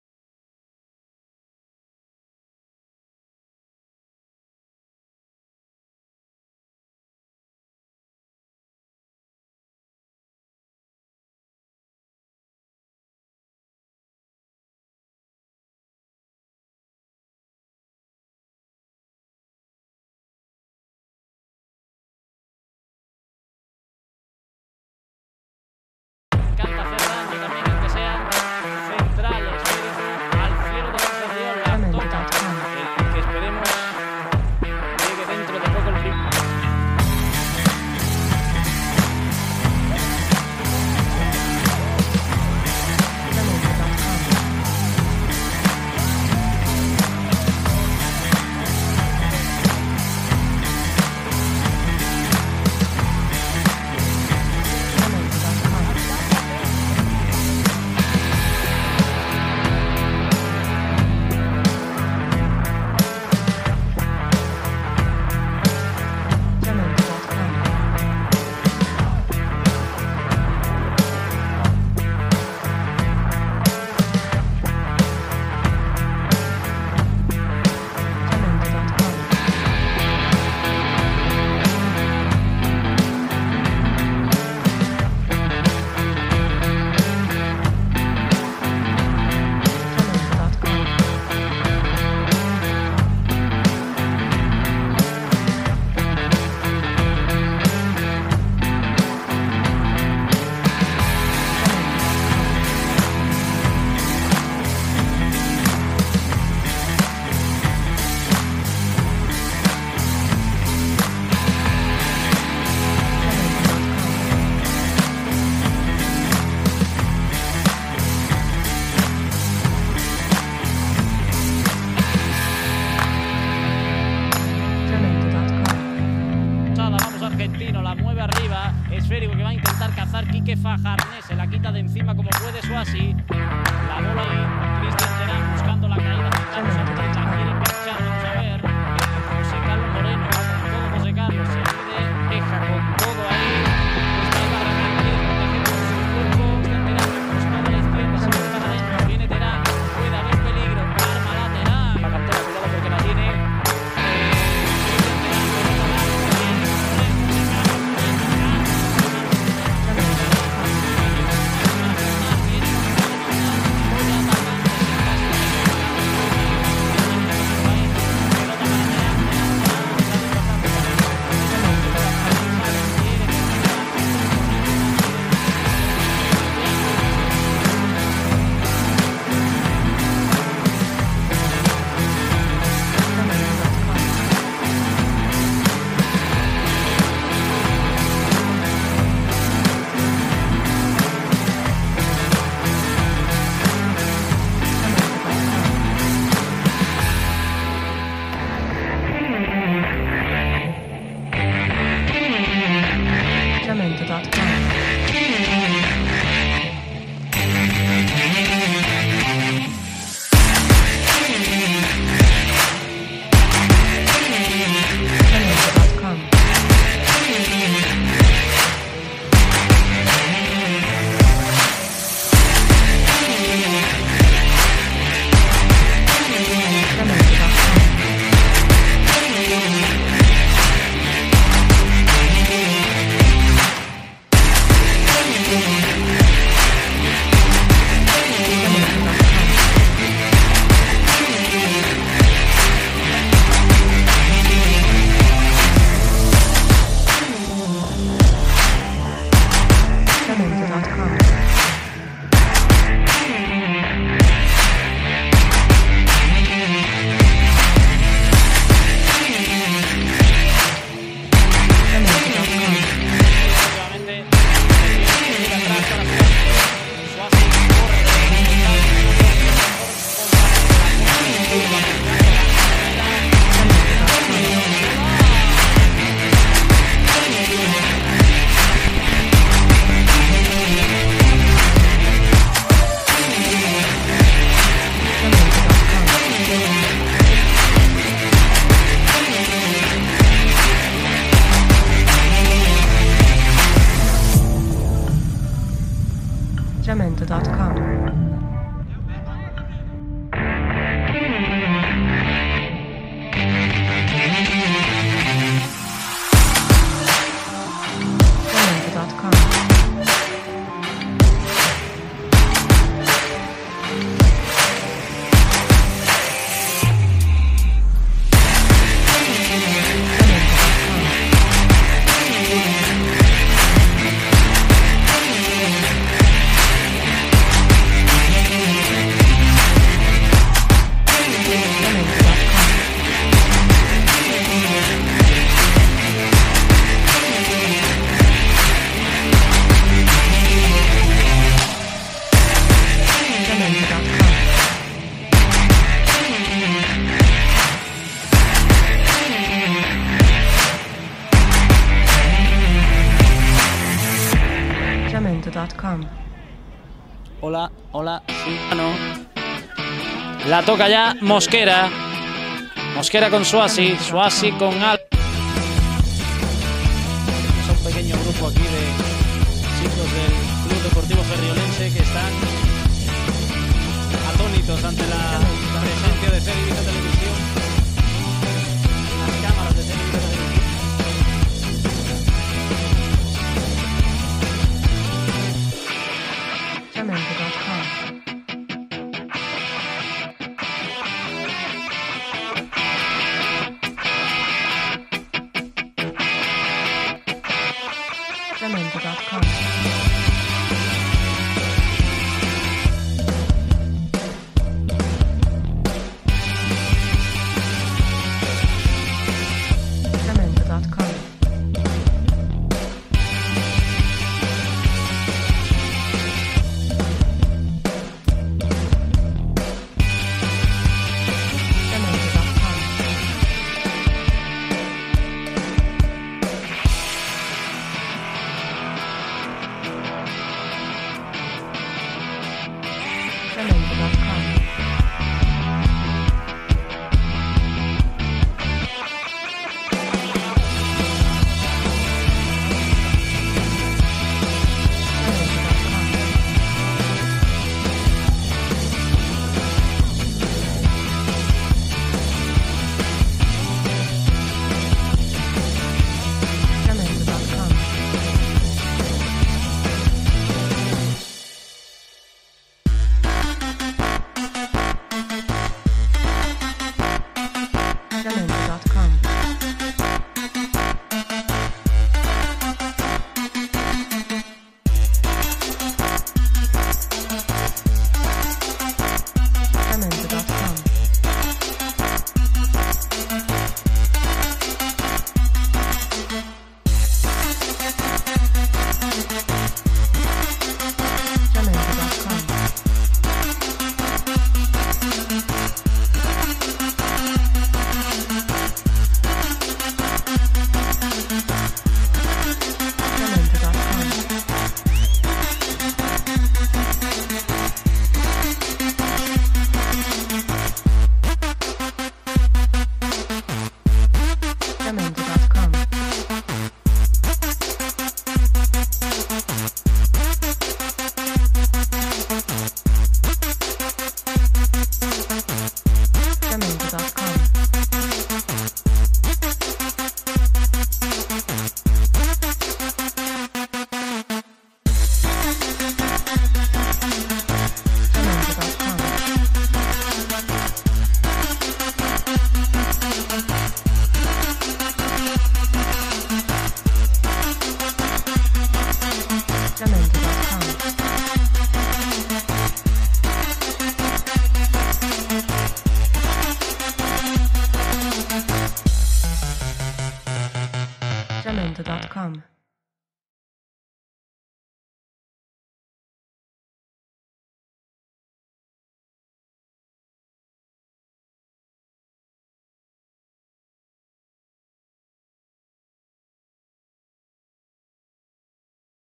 Juan Antonio Suasi - TOTAL PLAY3RS
Juan Antonio Suasi, defensa central del club deportivo Ferriolense de la Tercera División Balear. Destaca por el dominio del juego aéreo, la seguridad defensiva, una excelente salida de balón, un desplazamiento preciso y una excelente conducción para superar las líneas de presión del equipo rival.

Posición: Defensa Central.
Estatura: 182 cm.
Pierna dominante: Izquierda.
Club actual: C.D Ferriolense.
Categoría: Tercera División Balear.
Anteriormente: Juvenil División de Honor, Juvenil Nacional (ascenso a división de honor), Selección Balear Sub-16 y categorías inferiores del filial del R.C.D Mallorca.